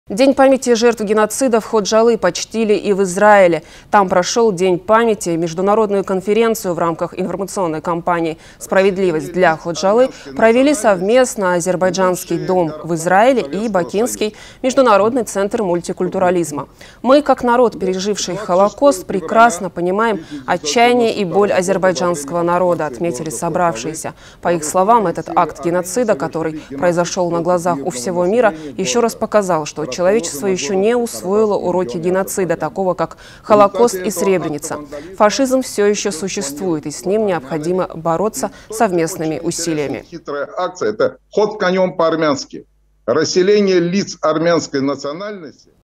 В Израиле прошел День памяти жертв геноцида в Ходжалы
День памяти жертв геноцида в Ходжалы почтили и в Израиле. Там прошел день памяти. Международную конференцию в рамках информационной компании «Справедливость для Ходжалы» провели совместно «Азербайджанский Дом в Израиле» и Бакинский Международный Центр Мультикультурализма. «Мы как народ, переживший Холокост, прекрасно понимаем отчаяние и боль азербайджанского народа» - отметили собравшиеся.  По их словам, этот акт геноцида, который произошел на глазах у всего мира, еще раз показал, что человечество еще не усвоило уроки геноцида, такого как Холокост и Сребреница. Фашизм все еще существует и с ним необходимо бороться совместными усилиями.